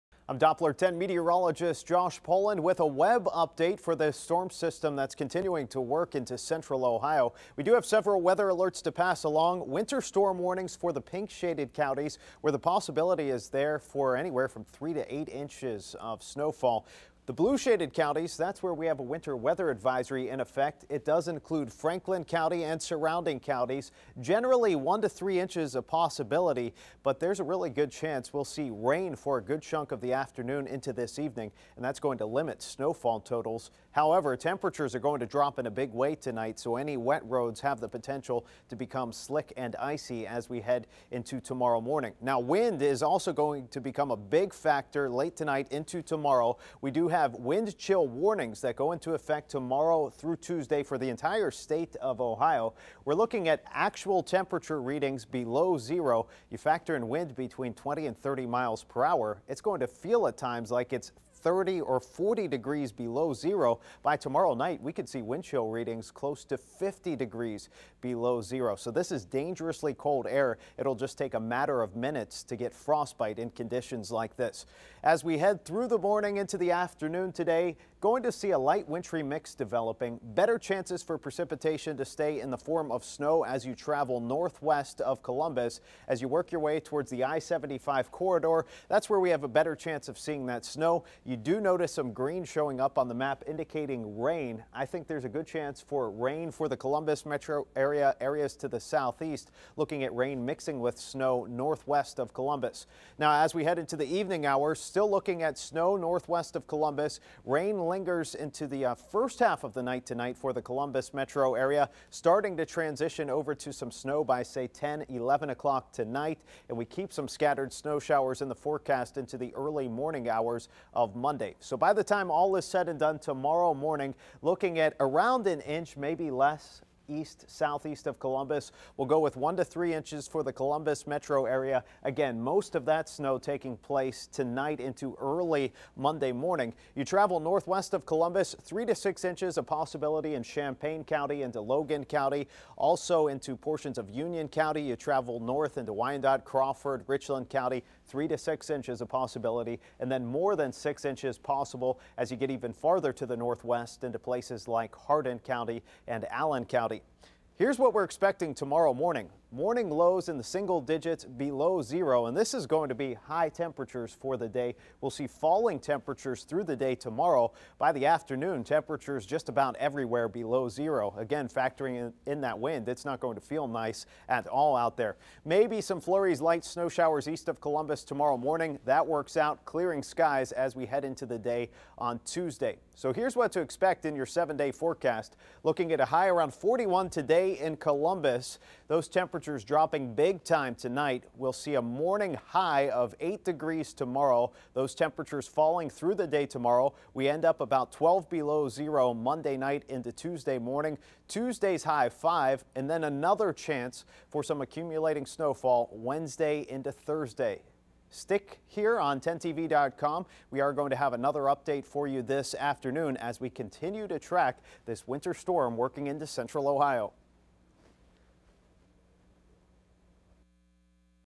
Winter Weather Update
Mostly cloudy with snow developing northwest of Columbus and a wintry mix possible for the Columbus metro area.